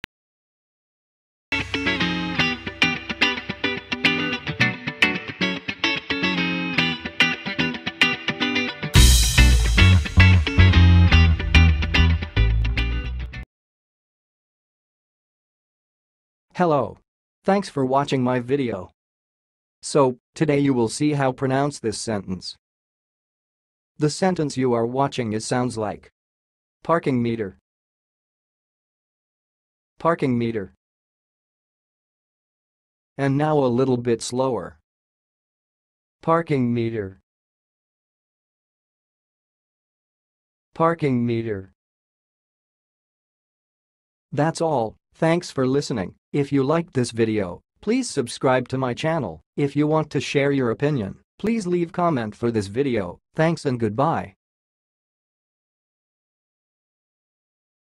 HOW TO PRONOUNCE PARKING METER?
PARKING METER - Getting Parking meter right: Pronunciation tips in 2023

A parking meter is a device used to regulate and charge for parking spaces in public areas.
It is typically a mechanical or electronic device that is placed near a parking space and requires users to insert coins, tokens, or prepaid cards to pay for the time they wish to park.
The meter displays the amount of time that has been paid for, and once the time expires, users are expected to move their vehicle or add more payment to extend their parking time.
Parking meters are commonly found in urban areas, parking lots, and public garages, and they serve as a source of revenue for municipalities while ensuring efficient use of parking spaces.

Pronunciation:
To pronounce 'parking meter', you can say it as 'par-king mee-ter' or 'par-kang mee-tur'.

Test yourself by speaking the following examples:
1. I need some change to feed into the parking meter
2. The parking meter only accepts credit cards
3. Make sure to check the time displayed on the parking meter
4. The parking meter ran out of time, and I got a parking ticket
5. The city has installed new smart parking meters